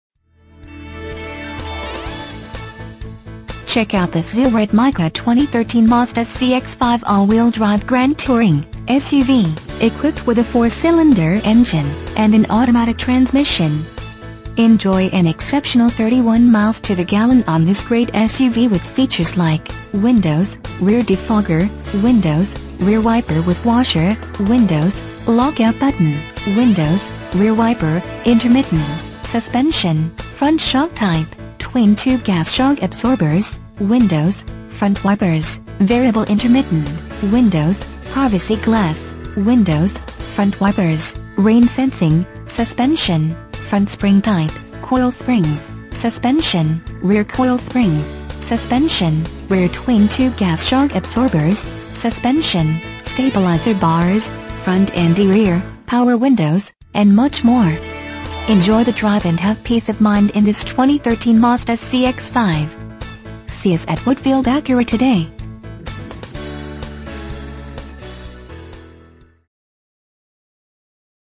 2013 Mazda CX-5 Woodfield Acura Hoffman Estates, IL 60195
Mazda CX Check out this Zeal Red Mica Mazda Cx-5 AWD Grand Touring 4dr SUV, equipped with a 4 Cylinder engine and an automatic transmission. Enjoy an exceptional 31 miles to the gallon on this great SUV with features like Windows, Rear Defogger, Windows, Rear Wiper With Washer, Windows, Lockout Button, Windows, Rear Wiper: Intermittent, Suspension, Front Shock Type: Twin-Tube Gas Shock Absorbers, Windows, Front Wipers: Variable Intermittent, Windows, Privacy Glass, Windows, Front Wipers: Rain Sensing, Suspension, Front Spring Type: Coil Springs, Suspension, Rear Coil Springs, Suspension, Rear Twin-Tube Gas Shock Absorbers, Suspension, Stabilizer Bars: Front And Rear, Power Windows, Exterior Mirrors, Heated, One-Touch Windows: 1, Daytime Running Lights, Rear Seats, Rear Heat: Vents, Warnings And Reminders, Tire Pressure Warning, Front Suspension Type: Macpherson Struts, Spare Wheel, Steel Rim Type, and much more. Enjoy the drive and have peace of mind in this Mazda Cx-5. See us at Woodfield Acura today!
MILES: 31,134
VIN:JM3KE4DE4D0106813

Woodfield Acura
1099 w. Higgins Rd.